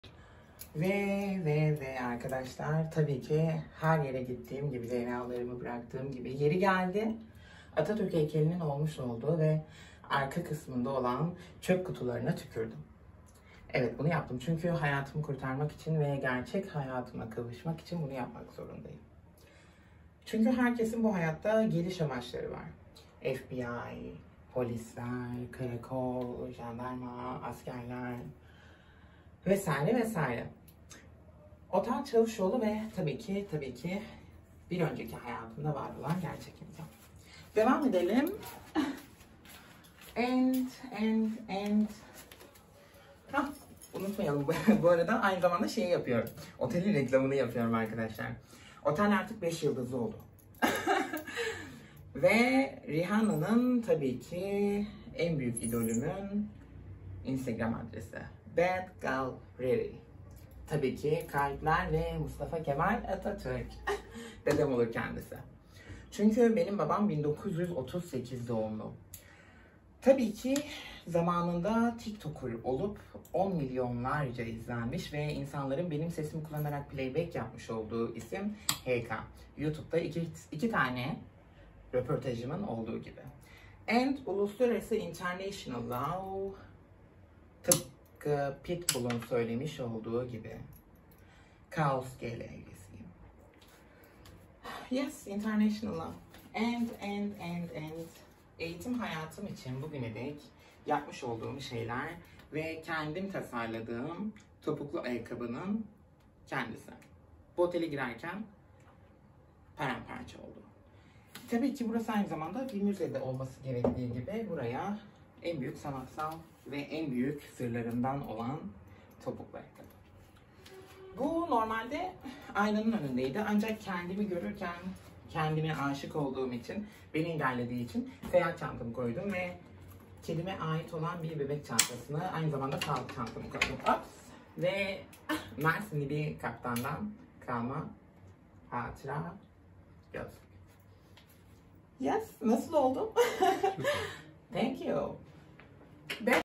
ÖZGEÇMİŞ PART 2
Adana’da international olan çavuşoğlu otele ilk defa gittim ve artık bi şeylerin başlangıcı olmalı dedim otelden atıldım 💕